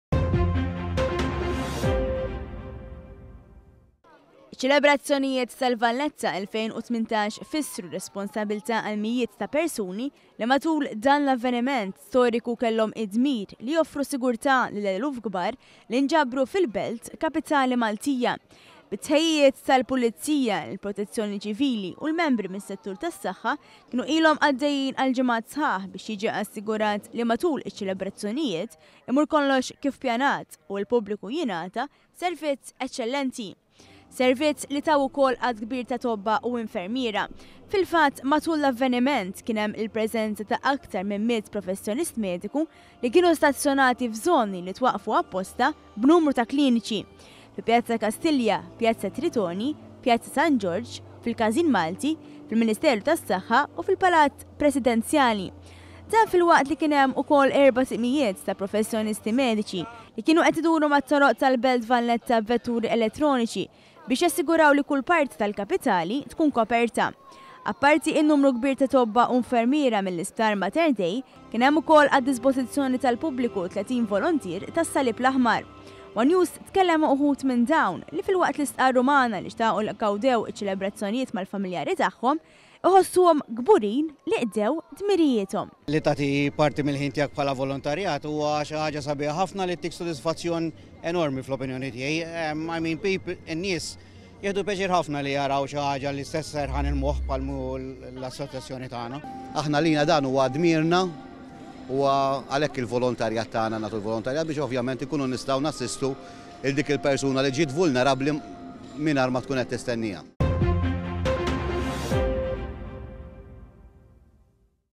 ĊELEBRAZZJONIJIET B’SIGURTÀ MASSIMA
ONE.COM.MT - Is-suċċess tal-ftuħ tal-Valletta elfejn u tmintax ma kienx ikun possibbli, mingħajr il-ħidma li għamlu mijiet ta’ persuni – li wara ħafna tħejjijiet assiguraw li tul l-ispettakli, jżommu l-ordni meħtieġa fil-Kapitali Maltija. Ħidma li saret b’ko-ordinazzjoni bejn il-Pulizija, il-Protezzjoni Ċivili, professjonisti mediċi u volontiera tas-salib l-aħmar. Ħidma li assigurat sigurtà massima tul iċ-ċelebrazzjonijiet.